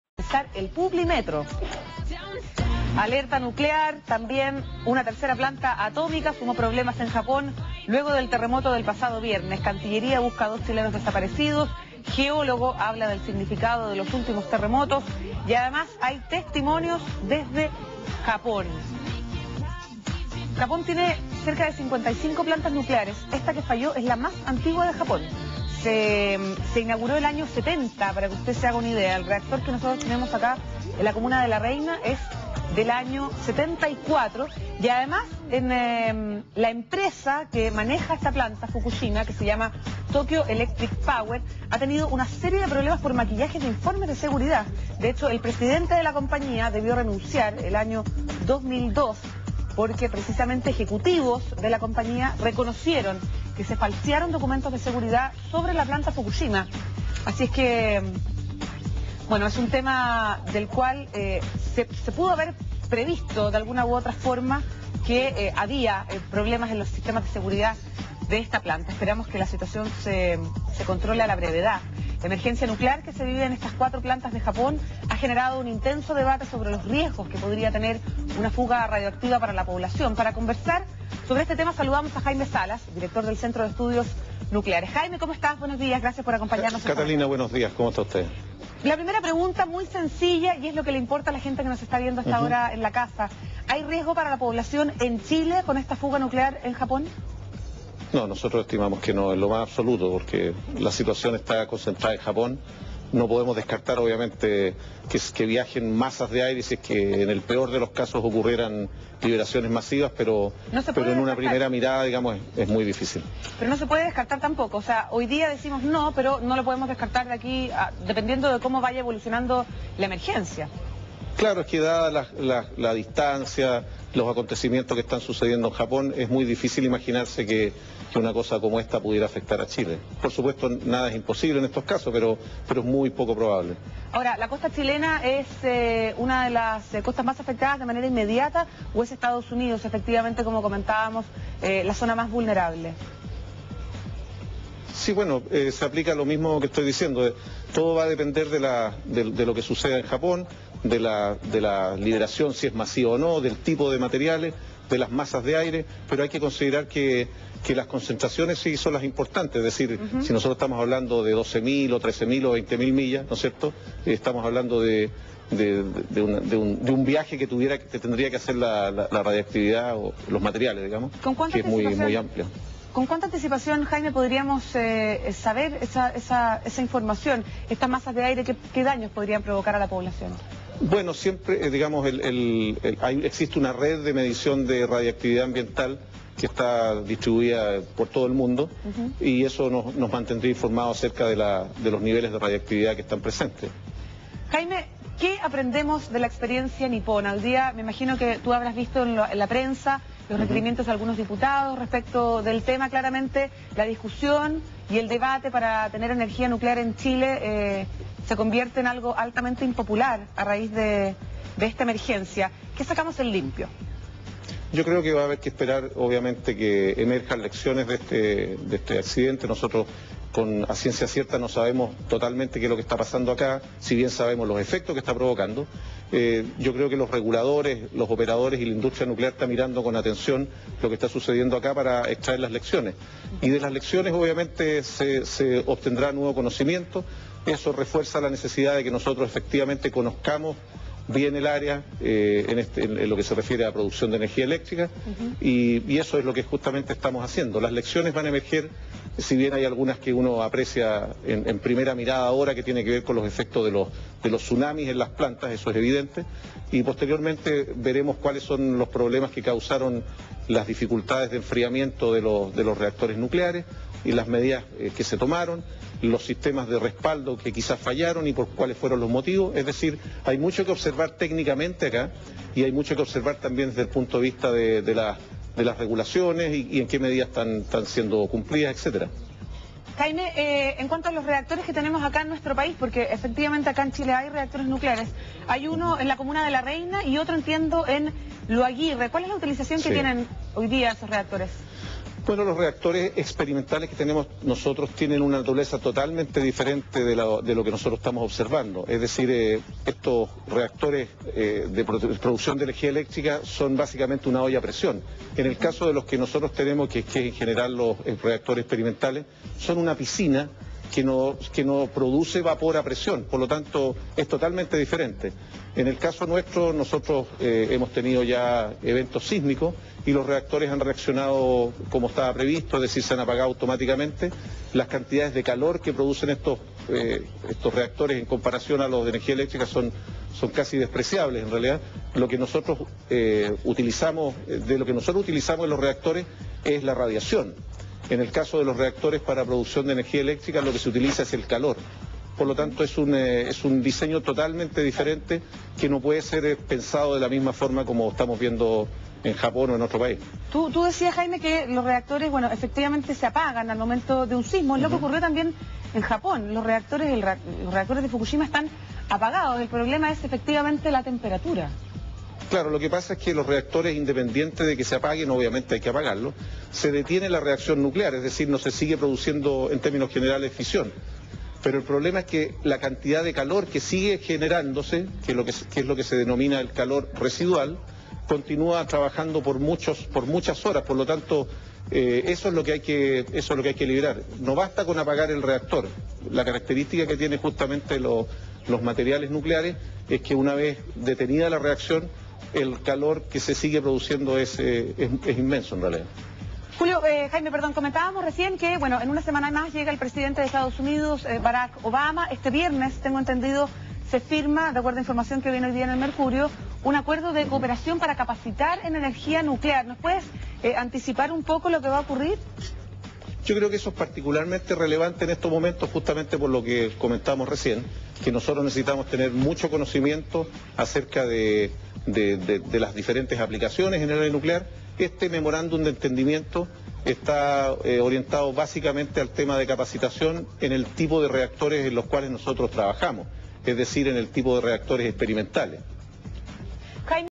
Jaime Salas, Director Ejecutivo de la CCHEN en Mega Noticias
Posterior a la crisis nuclear en Japón se abre el debate de la implementación de tecnologías nucleares de potencia en Chile.

Es por eso que el vocero y Director Ejecutivo de la Comisión Chilena de Energía Nuclear, Jaime Salas, aclara todas las dudas existentes respecto al tema y explica cuales son las funciones de la CCHEN y sus medidas de seguridad.